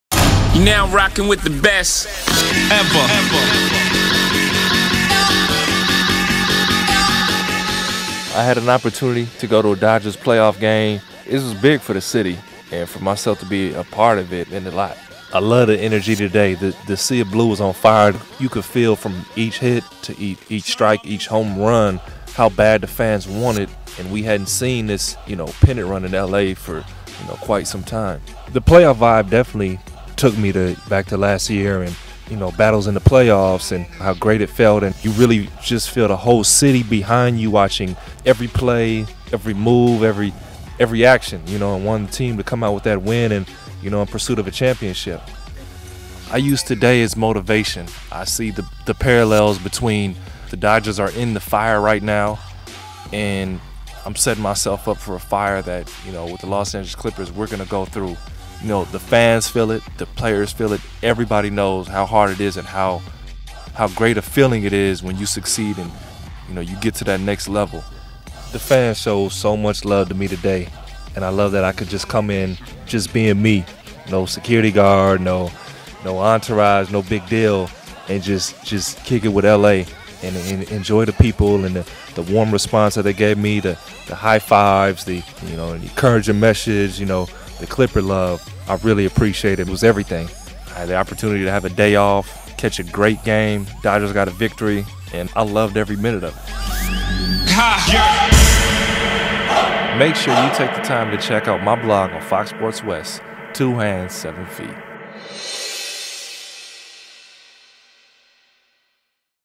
Ryan Hollins at Game 5 of the NLCS // Dodgers Vs. Cardinals
After a three-game road trip to Sacramento, Utah and Phoenix‎, I had the chance to take a break and go to Game 5 of the NLCS and watch the Dodgers get a much-needed win.